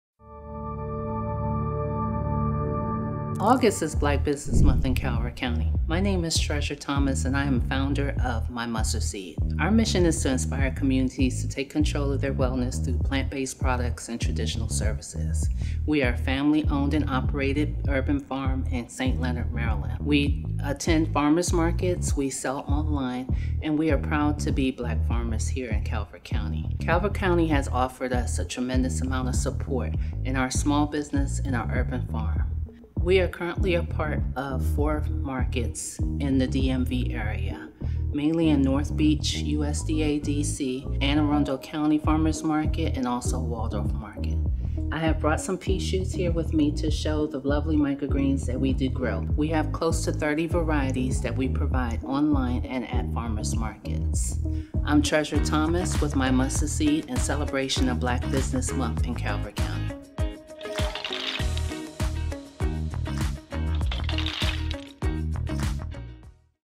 Black Business Month - My Mustard Seed
In continuation of our celebration of National Black Business Month, meet My Mustard Seed owner, Tresor Thomas. Thomas created My Mustard Seed, a company focused on organic, non-GMO microgreens to inspire communities to take control of their wellness through plant-based products and traditional services.

Based in St. Leonard, Thomas specializes in micro-arugula, broccoli, cress, kale, kohlrabi, and mizuna as part of the Farm to Table movement. Learn more about their mission and products